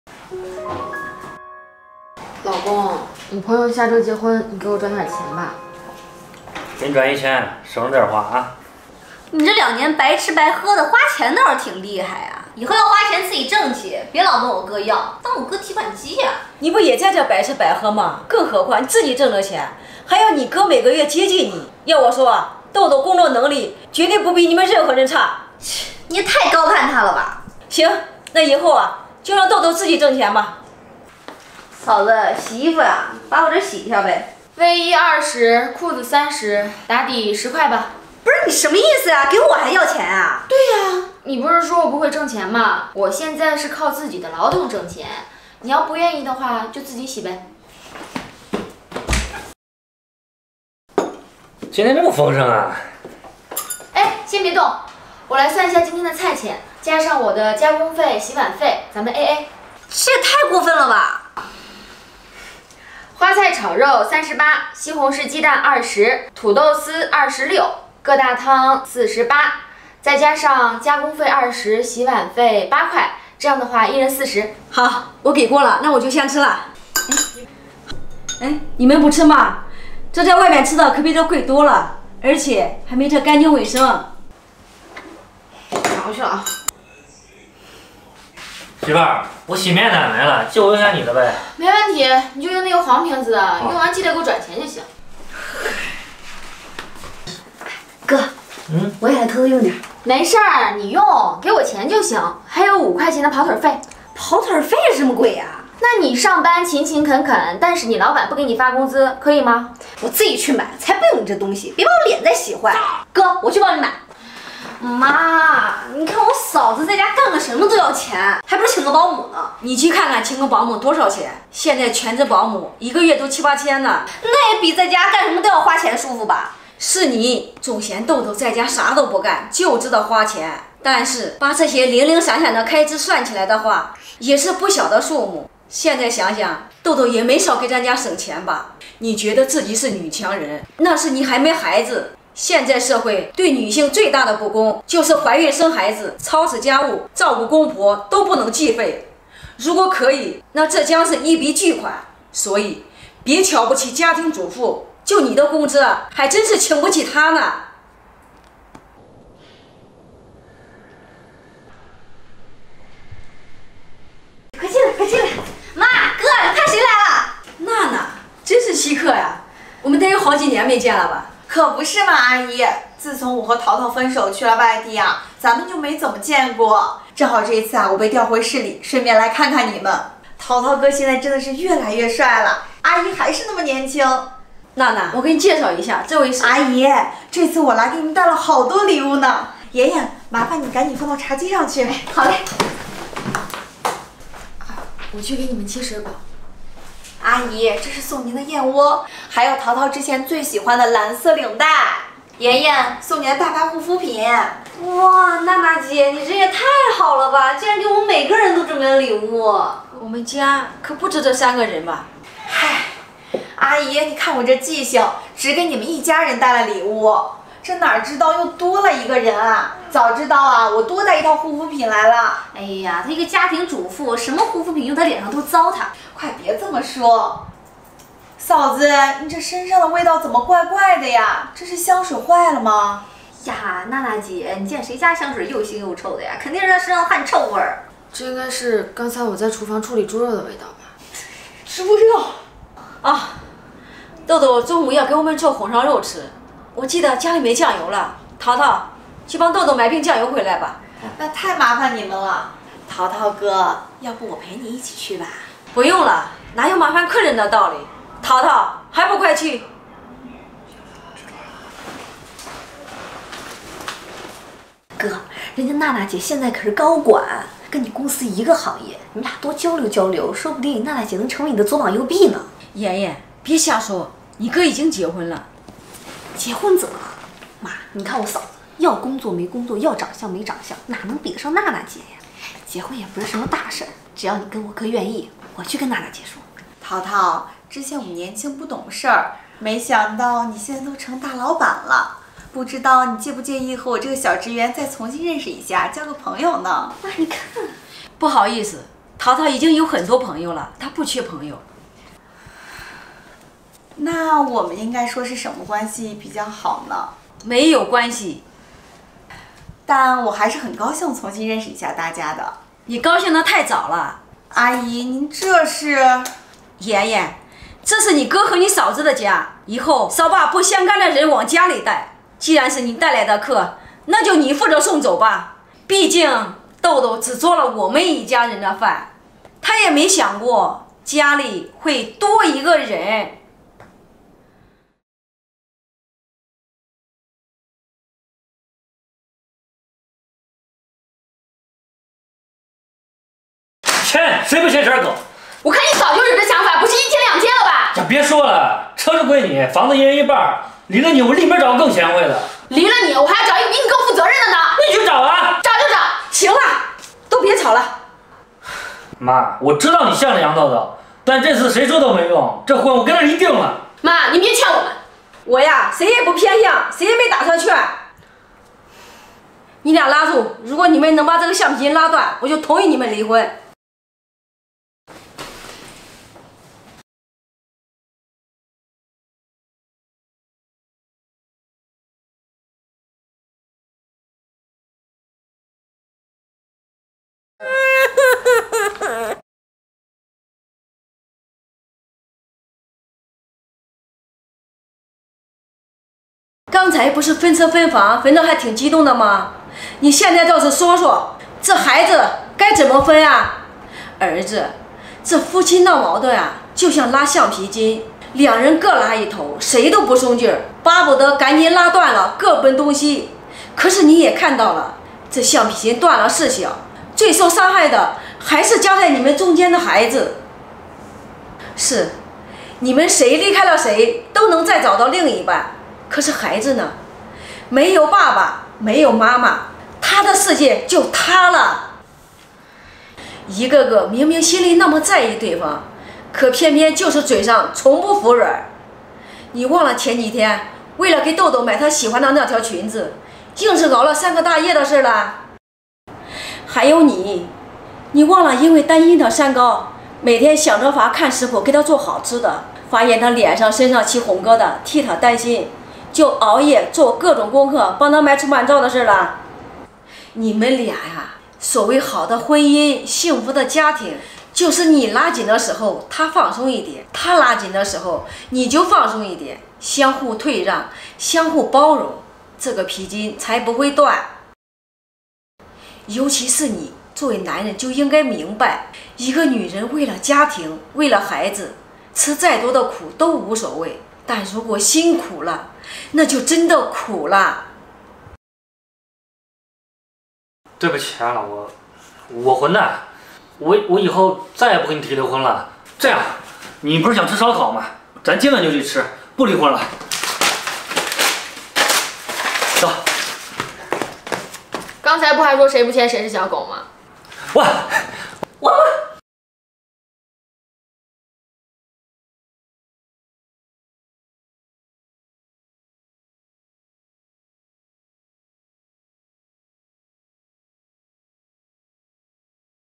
小姑嫌棄媳婦在家白吃白喝，婆婆給媳婦想了個妙招，把小姑治的服服帖帖的【婆婆也是妈】
請放心，本視頻是一部原創拍攝的視頻，沒有任何不良行為。
大家好，这里是一对婆媳
我们是逗逗和婆婆
这是在YouTube唯一账号
主要分享婆媳间的欢笑与感动
传递世间美好